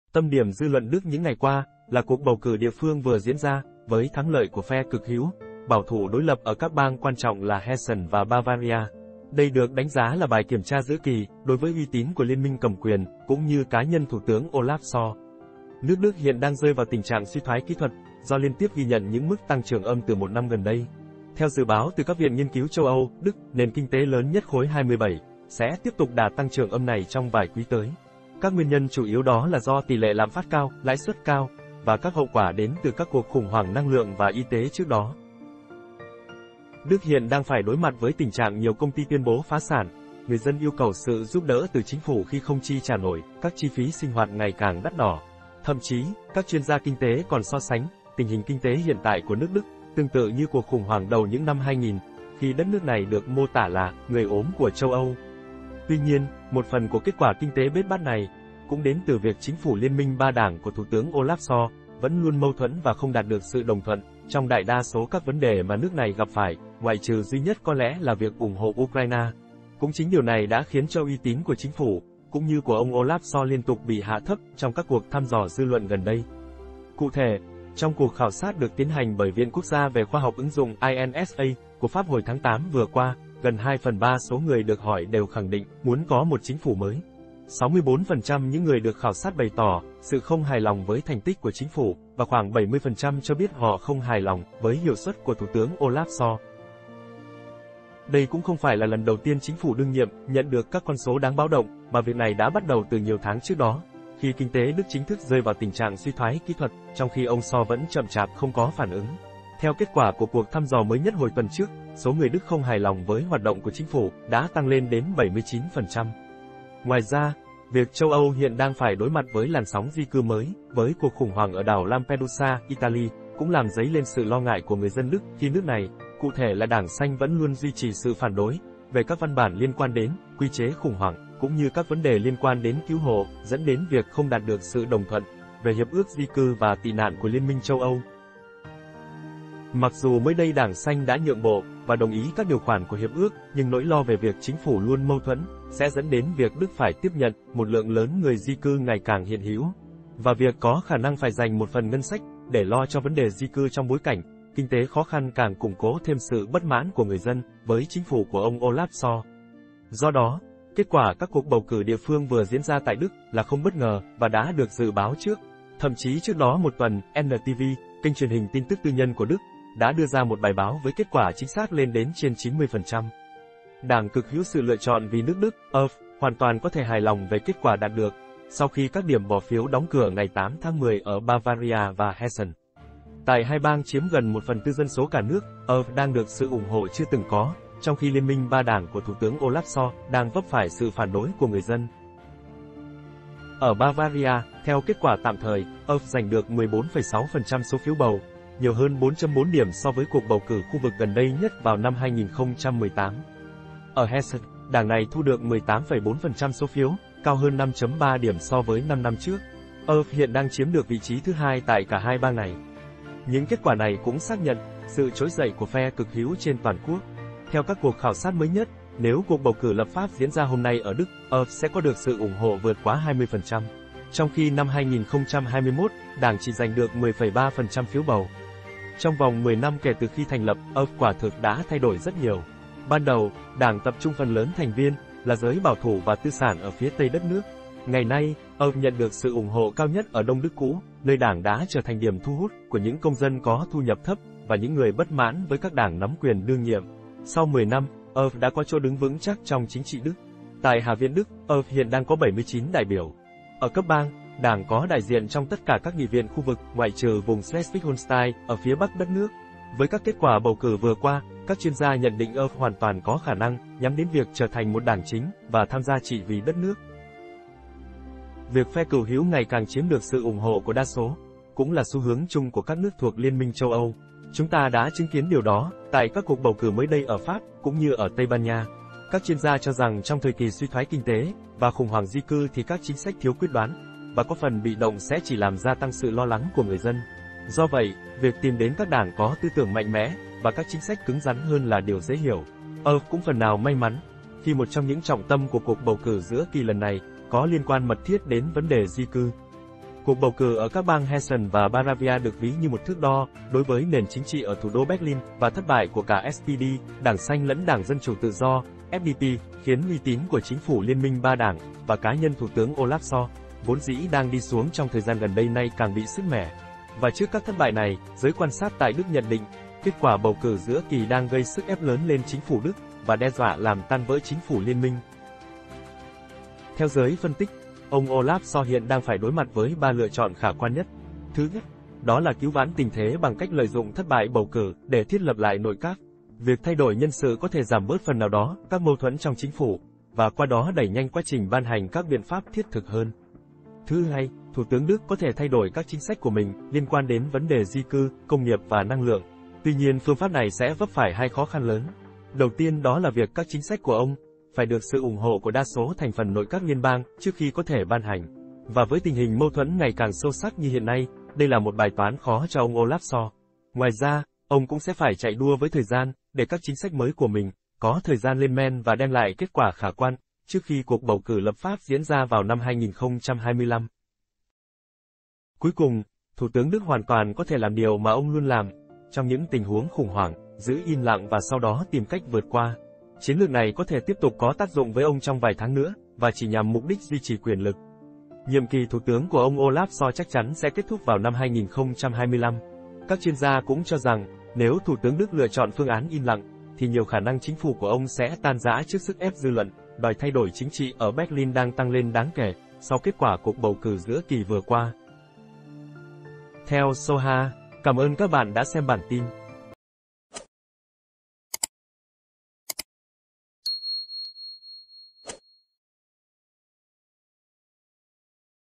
PHE CỰC HỮU TẠI ĐỨC ĐE DỌA VỊ TRÍ LIÊN MINH CẦM QUYỀN, CŨNG NHƯ THỦ TƯỚNG OLAF SCHOLZ
Tâm điểm dư luận Đức những ngày qua, là cuộc bầu cử địa phương vừa diễn ra, với thắng lợi của phe cực hữu, bảo thủ đối lập ở các bang quan trọng là Hessen và Bavaria. Đây được đánh giá là bài kiểm tra giữa kỳ, đối với uy tín của liên minh cầm quyền, cũng như cá nhân Thủ tướng Olaf Scholz.